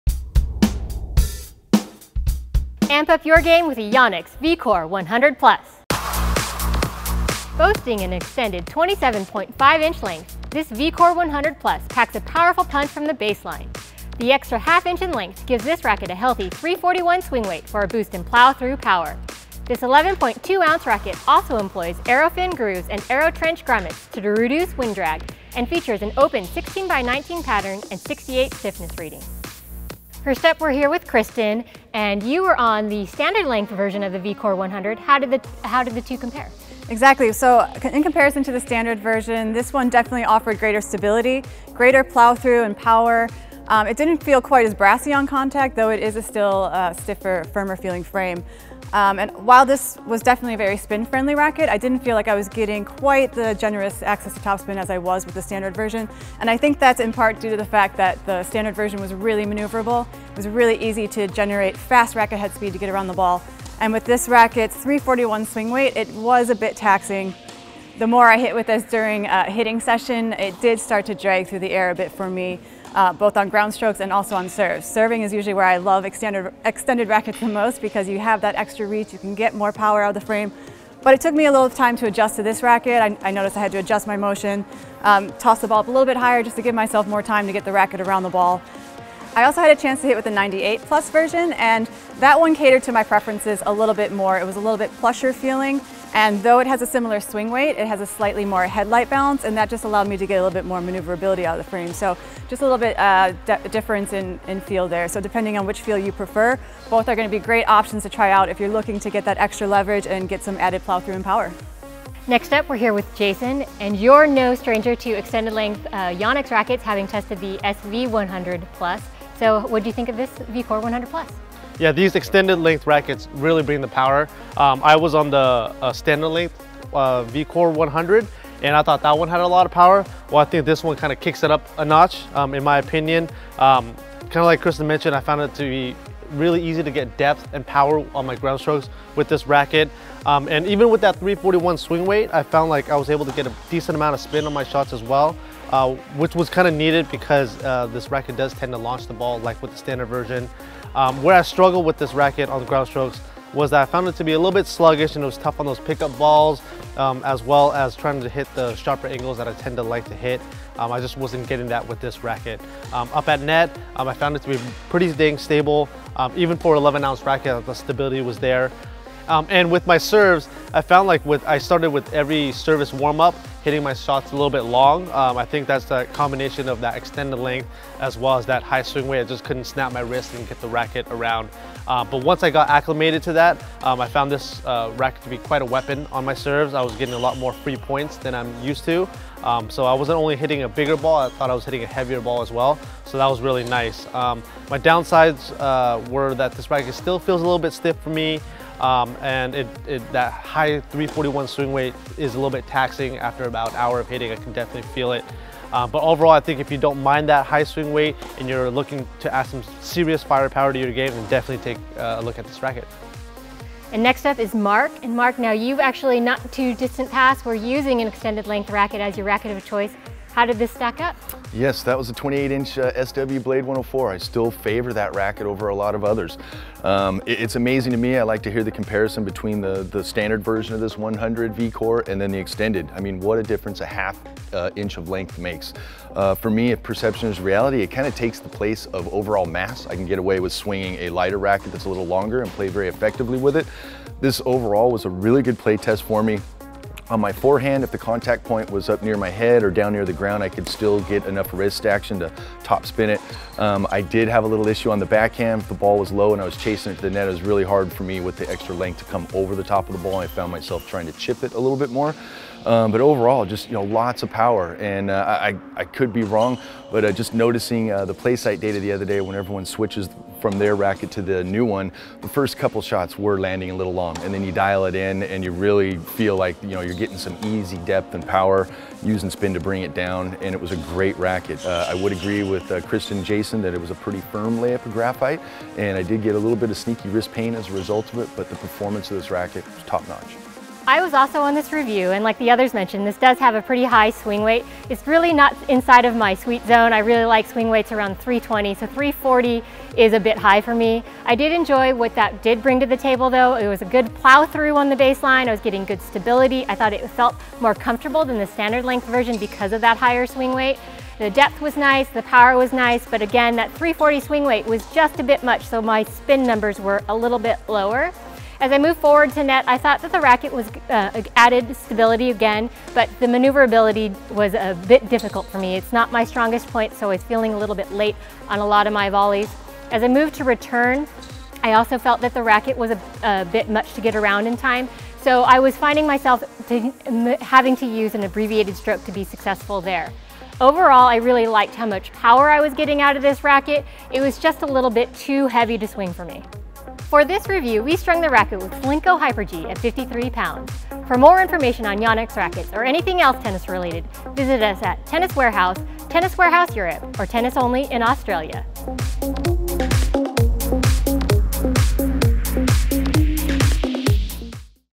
Yonex VCORE 100+ (Plus) Tennis Racquet Review
tenniswarehouse yonextennis Yonex VCORE 100+ (Plus) Tennis Racquet Review

Updated with extra spin and power, this extended length racquet is ideal for strong intermediates and advanced players.

Power, pop and spin! See how our playtesters liked the racquet that replaces the Yonex VCORE SV 100+.